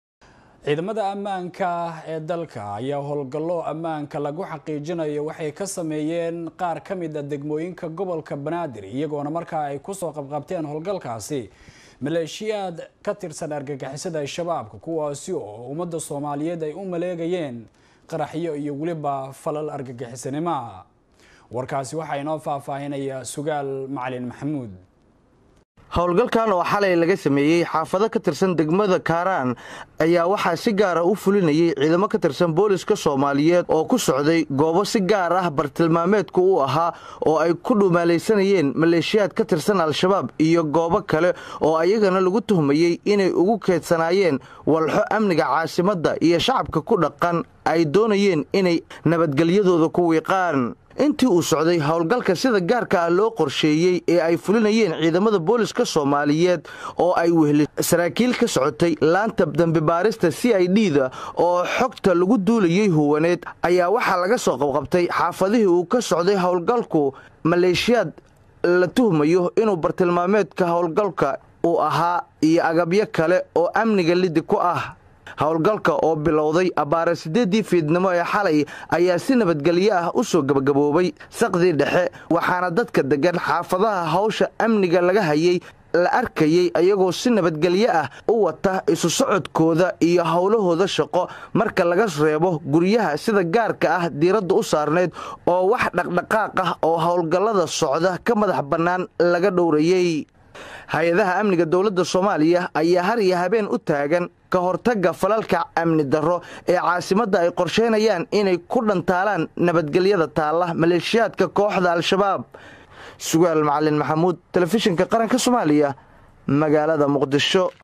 Nisa
Hay’adaha amniga dowlada Soomaaliya ayaa har iyo habeen u taagan ka hortaga falalka amni darada oo ay qorsheynayaan Nabad diidka Al shabaab oo baryihii ugu dambeeyay laga fashiliyay falal qaraxyo ah oo ay doonayeen inay ku waxyeelaan shacabka Soomaaliyeed.